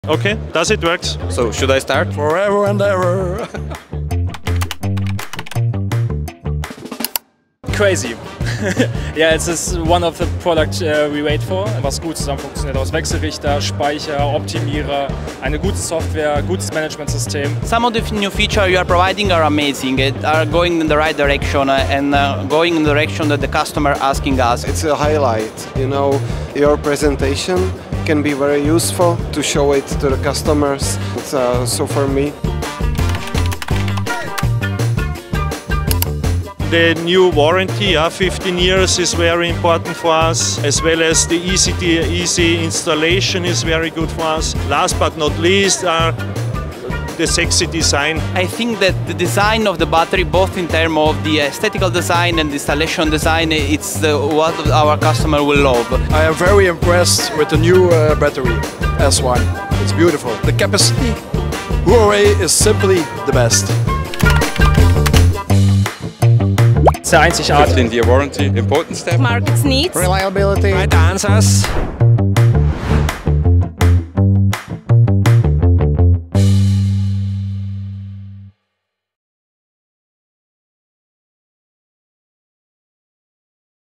LIMITLESS1 Applause: How Huawei LUNA S1 Impresses Industry Experts
🌟 Explore the cutting-edge features of Huawei's groundbreakingLUNA S1 in our latest video! Join industry experts as they delve into its innovative design, unparalleled performance, and game-changing impact on sustainable energy solutions. Don't miss out on this exclusive insight into the future of energy technology! Discover the limitless potential of HuaweiLUNAS1. LimitlesS1 💡🚀